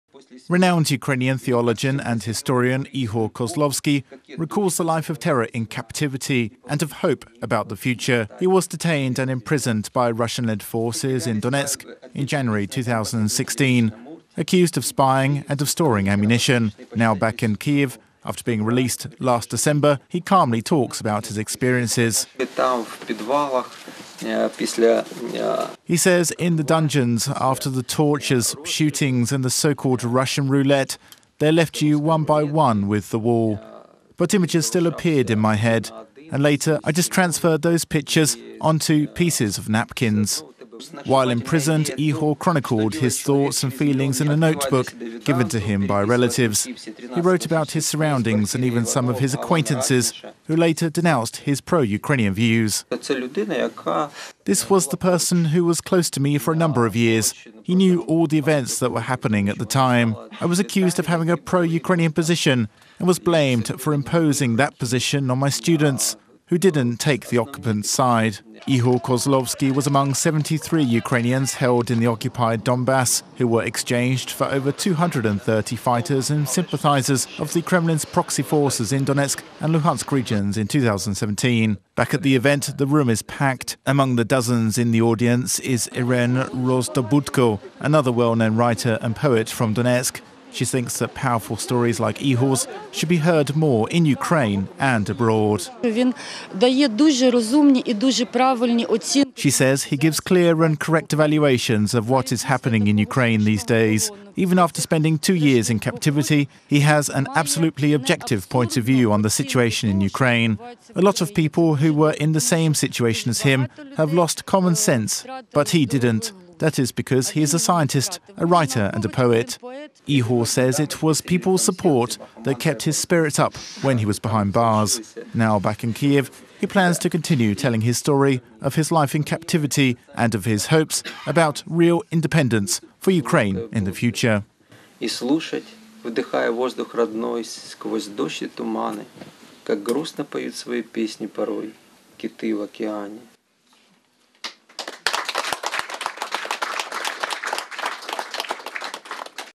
Evening With Ihor Kozlovsky: Poetry in Captivity And Dreams About Freedom
Renowned Ukrainian historian Ihor Kozlovsky survived through two years in the captivity of Russian led forces in Donbas.
After regaining his freedom, the poet organizes meetings with other people where he shares his experience and his thoughts on the situation in Ukraine.